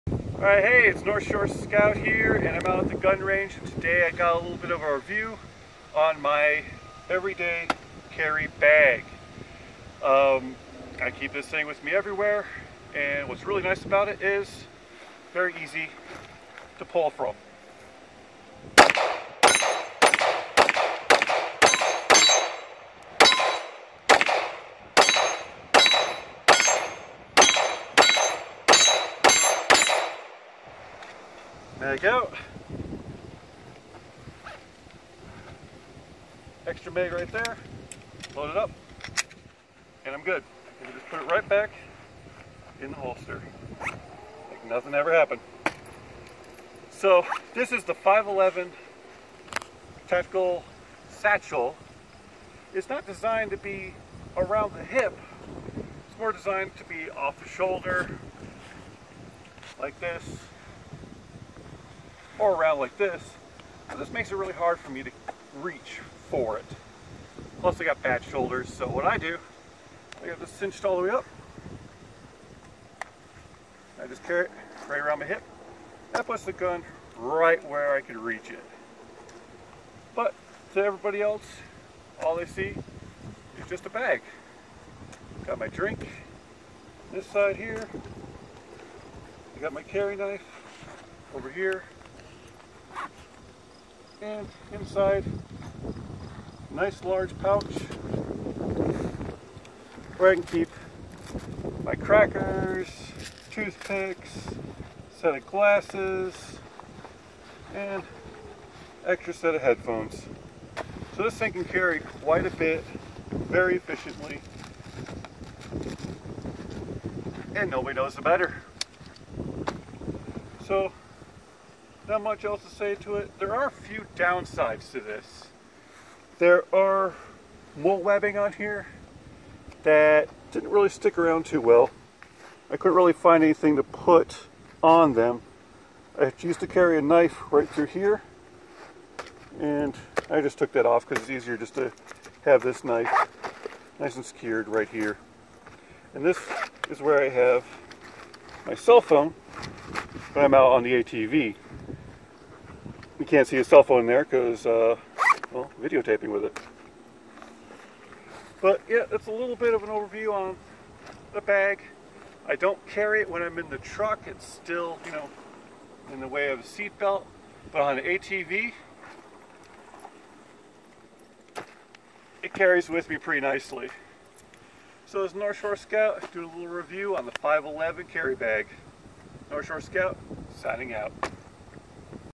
5.11 Push Pack review
non sponsored video. this is a bag i got years ago and has been my favorite way to carry my everyday things!